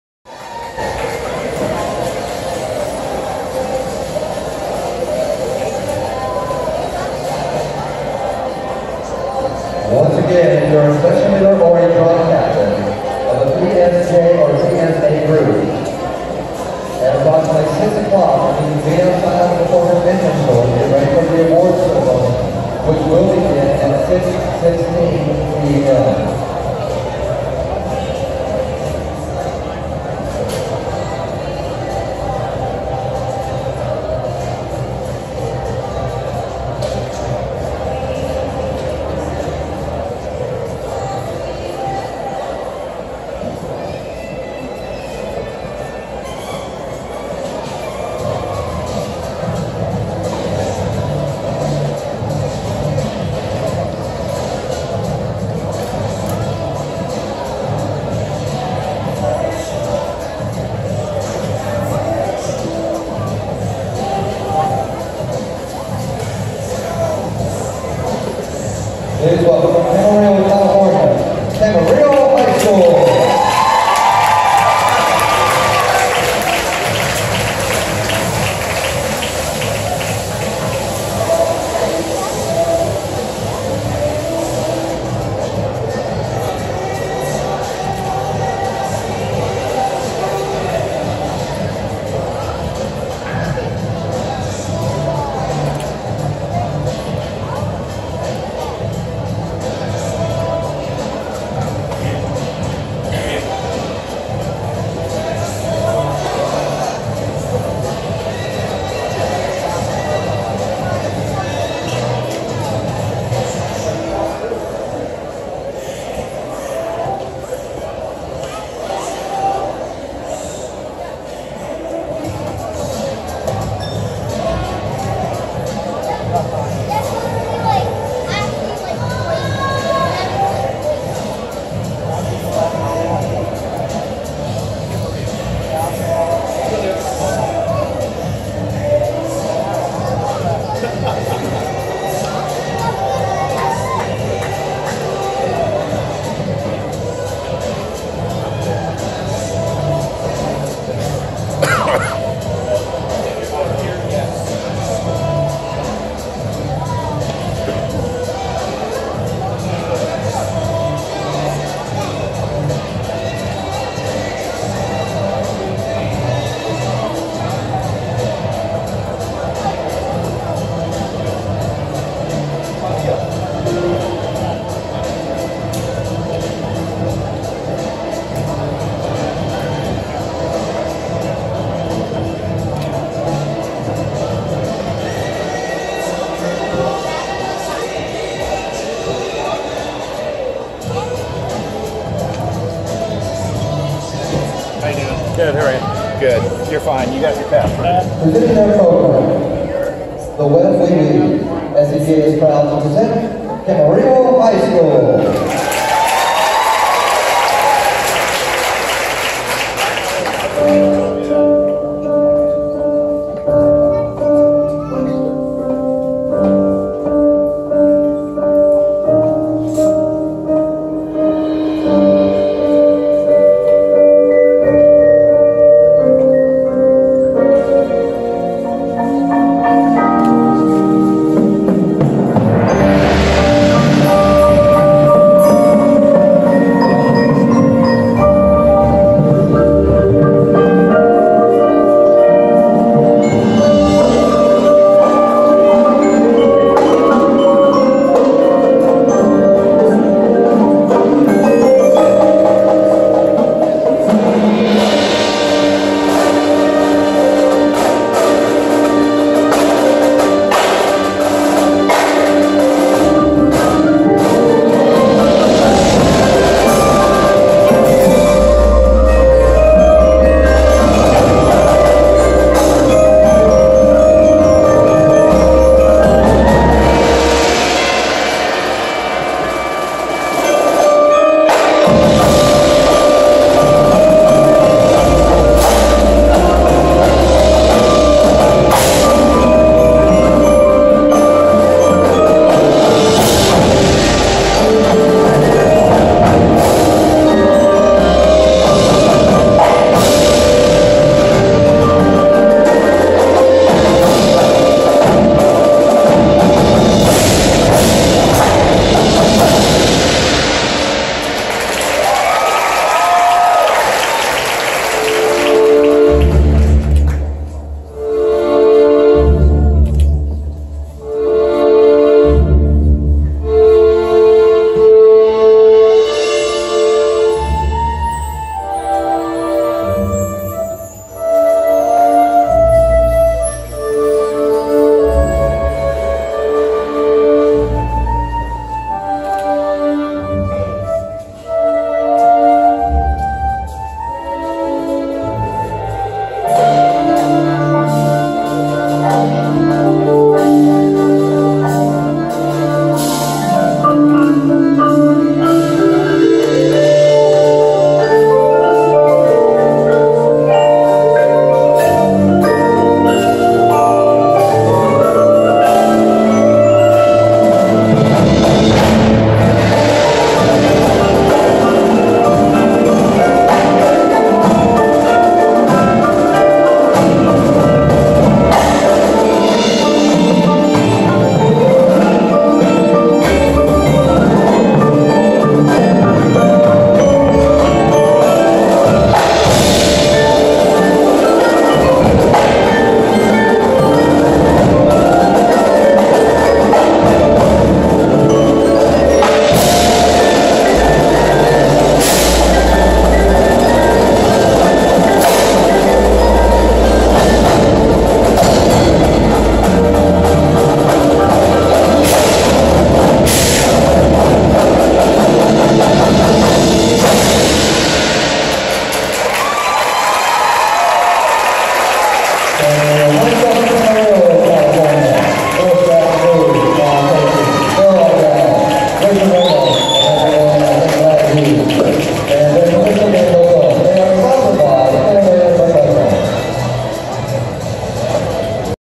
ACHS Winter Drumline Performance - Monrovia High School - 3/4/17
Sorry for the jittery video in spots. Performance starts at 3:20